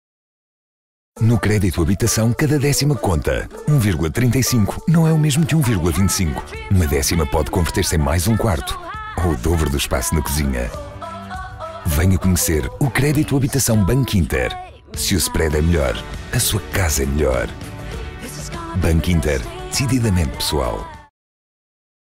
Spot Bankinter Portugal Hipoteca: 'Si tu hipoteca es mejor, tu casa es mejor'
Después del lanzamiento de marca, os presentamos la primera campaña de producto de Contrapunto BBDO y Proximity Madrid para Bankinter Portugal. Es una campaña mutimedia para ‘Credito Habitaçâo’ (Hipotecas) donde contamos que si tu hipoteca es mejor, tu casa es mejor.